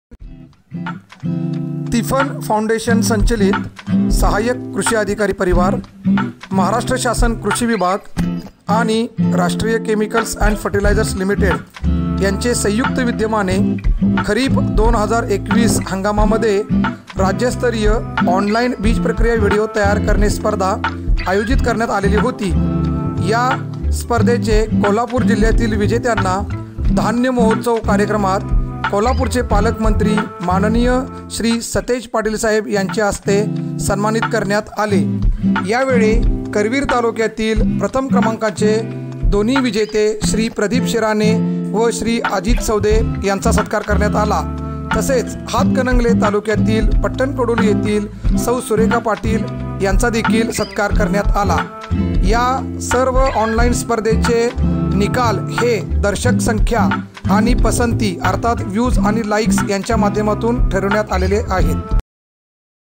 बीजप्रक्रिया स्पर्धा पारितोषिक वितरण समारंभ | The FarmBook
ऑनलाइन बीजप्रक्रिया स्पर्धा पारितोषिक वितरण जिल्हा कोल्हापूर. मा. पालकमंत्री श्री सतेज पाटील साहेब यांचे शुभहस्ते